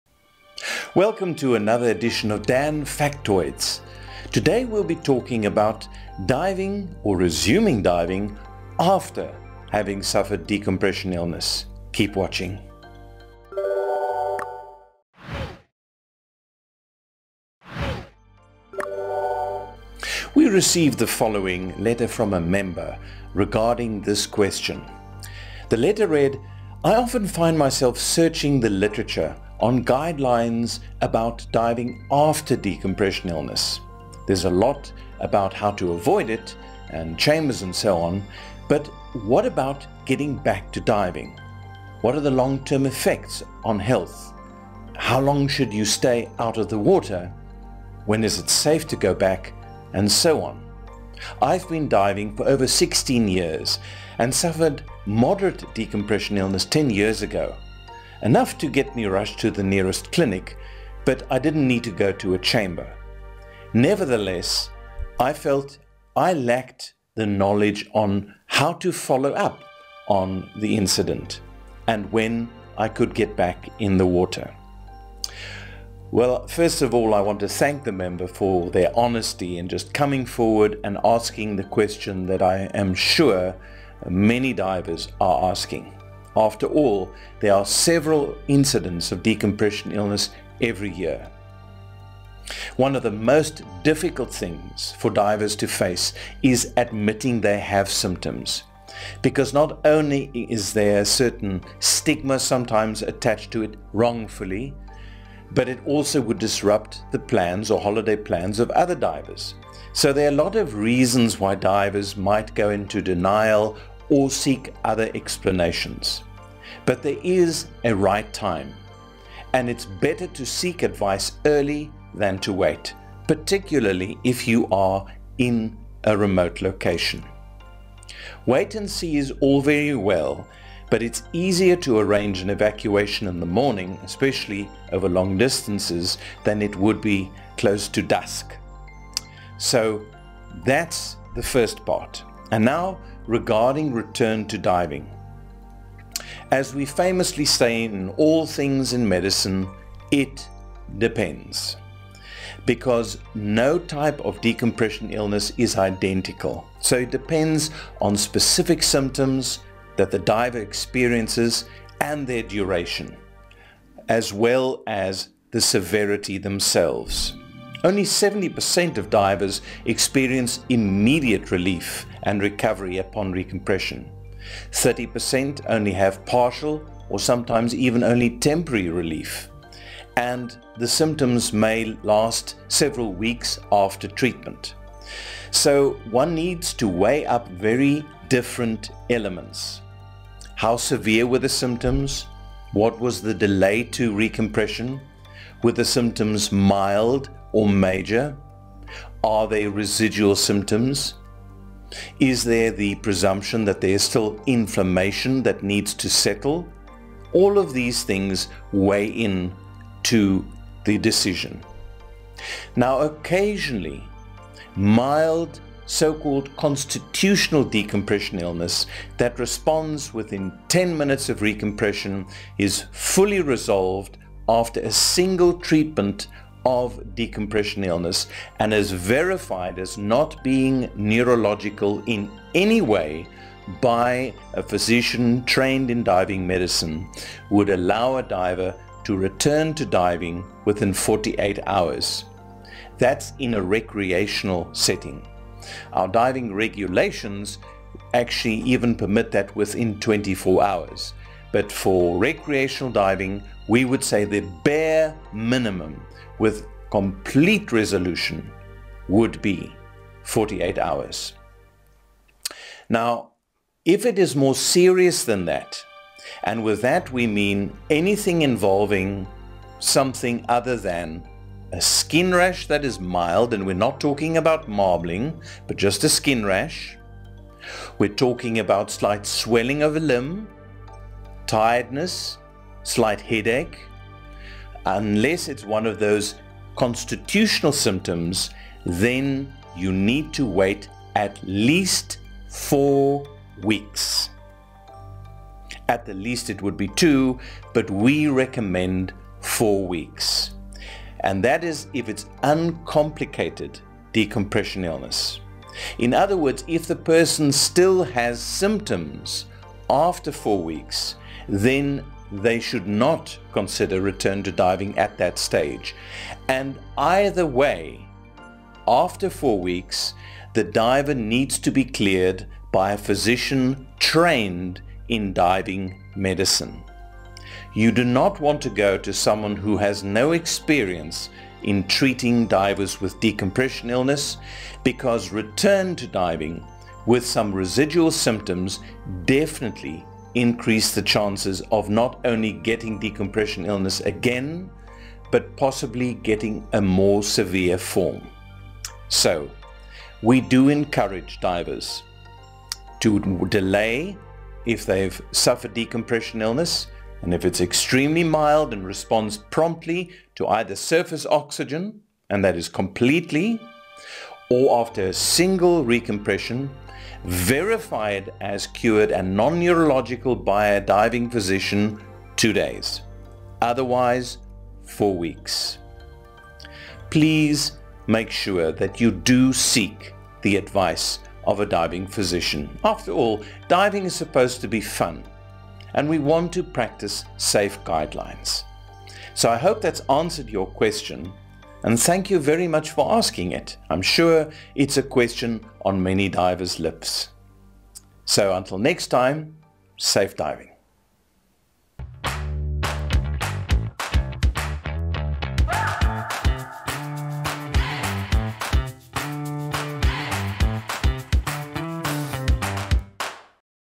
Diving After Decompression Sickness
Welcome to another edition of the 5-minutes factoids series. In this video Dr. Frans Cronje talks about diving after a decompression sickness incident.